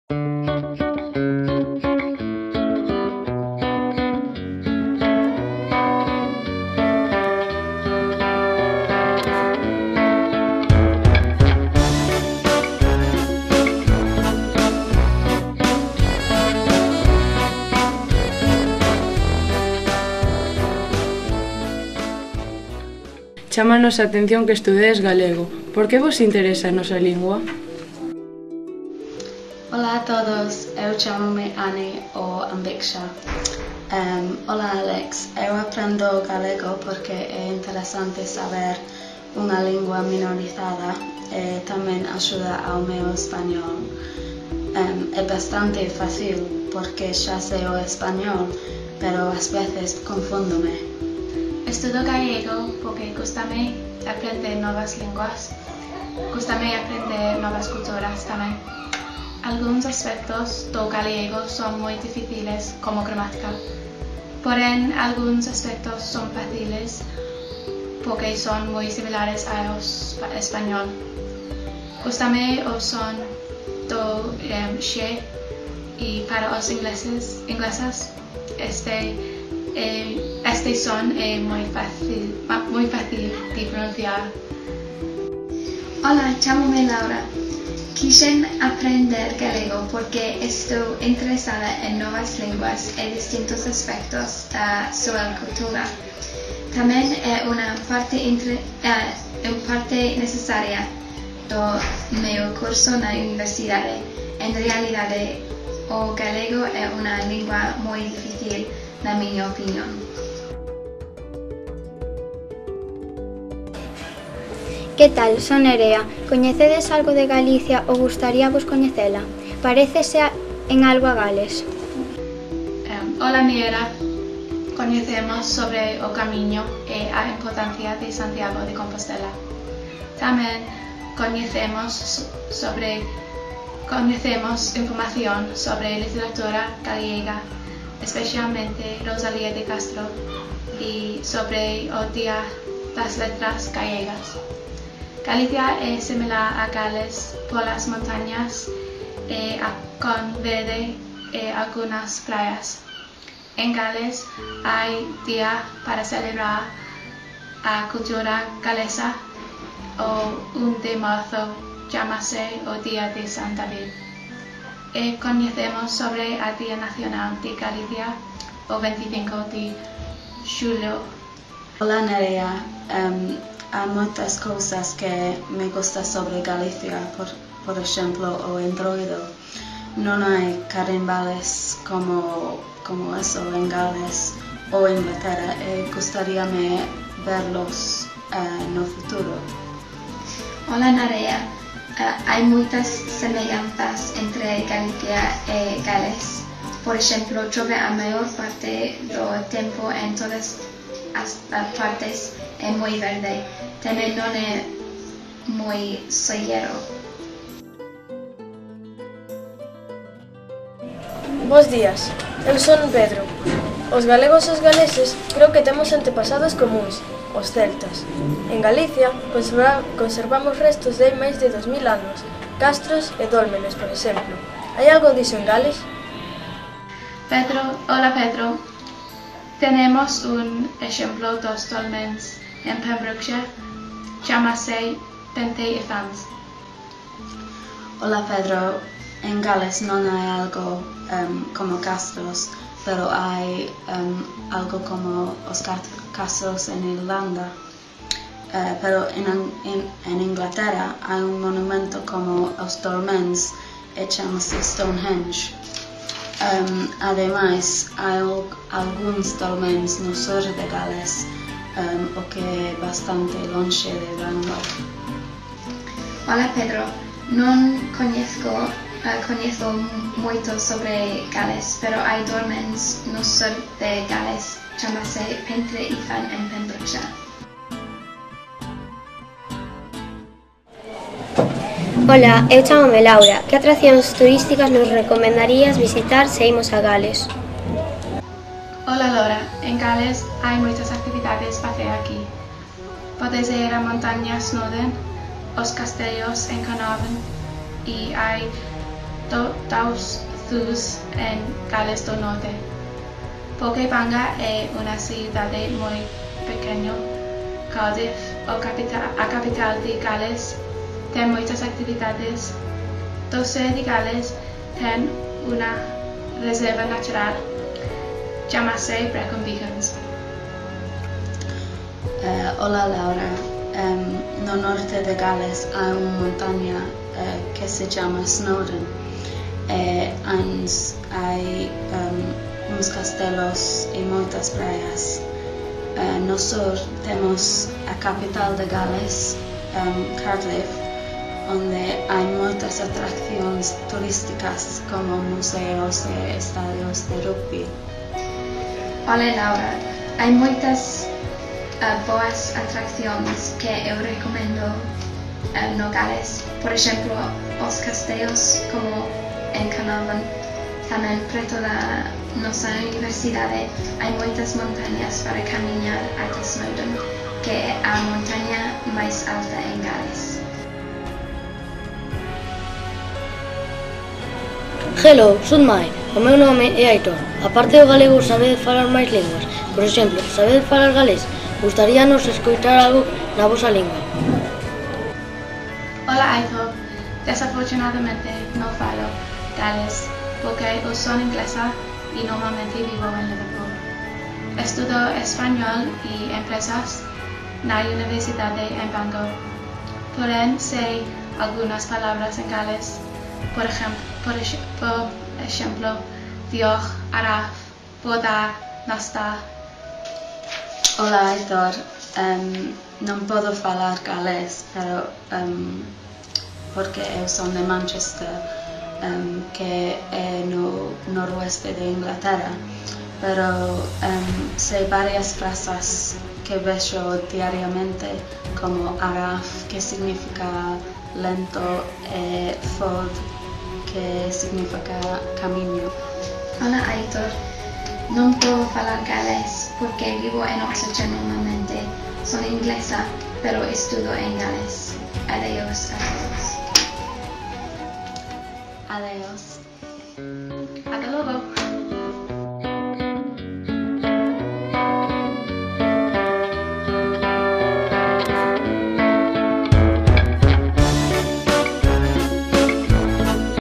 FIESTRA ABERTA CONVERSAMOS CON ESTUDANTES GALEGO EN GALES
María, Nerea, Pedro, Laura e Aitor conversan con estudantes de galego na Universidade de Bangor, no País de Gales